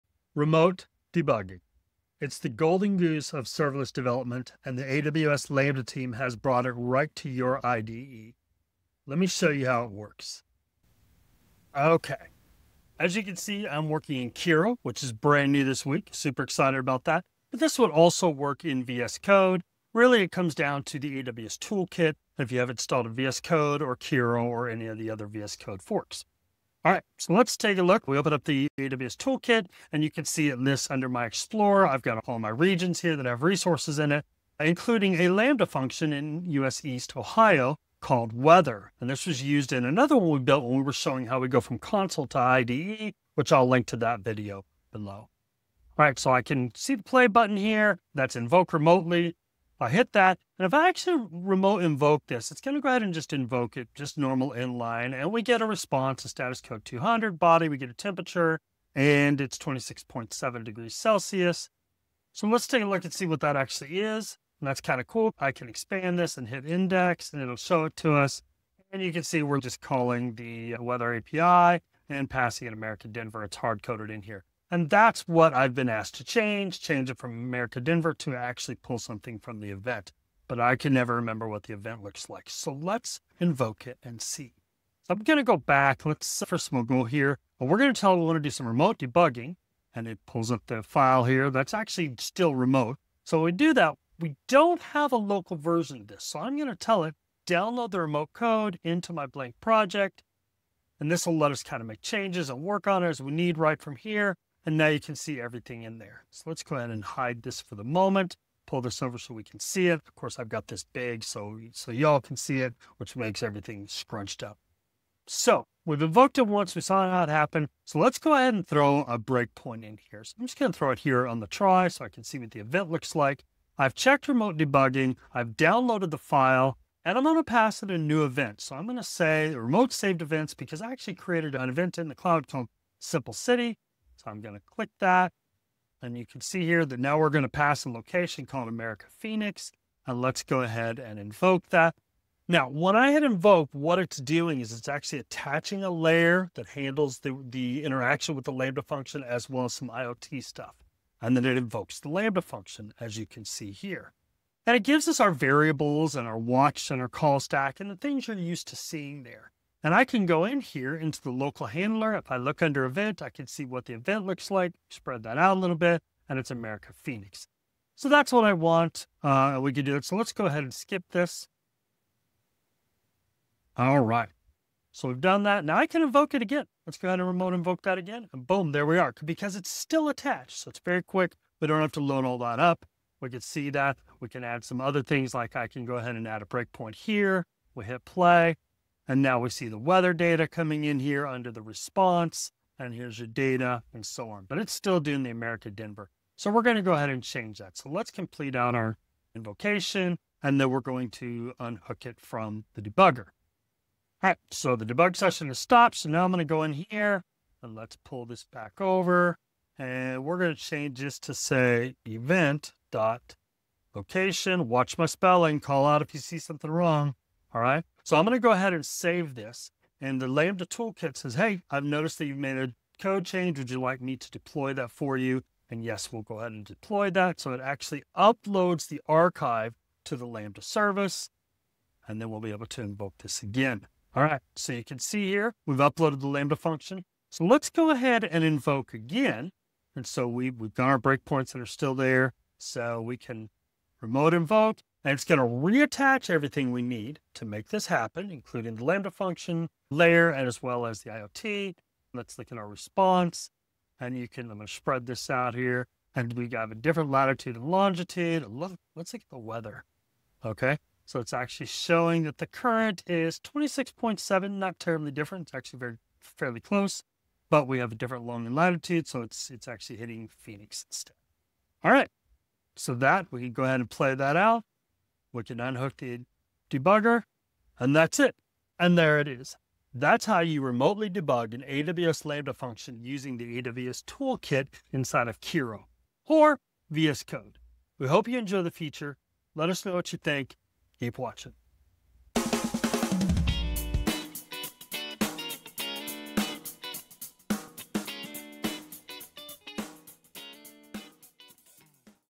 Remote Debugging AWS Lambda with AWS Toolkit in Kiro/VS Code
Learn how to perform remote debugging on AWS Lambda functions using the AWS Toolkit in Kiro or VS Code. In this tutorial, we'll walk through setting up the toolkit, invoking remote functions, adding breakpoints, and editing code. We'll also demonstrate the process of downloading remote code, attaching debuggers, and deploying changes. Don't miss this comprehensive guide to mastering serverless development with AWS!

00:00 Introduction to Remote Debugging
00:28 Exploring the AWS Toolkit
00:45 Invoking and Debugging Lambda Functions
01:25 Modifying and Testing Code
03:39 Deploying Changes to Lambda
03:54 Final Debugging and Conclusion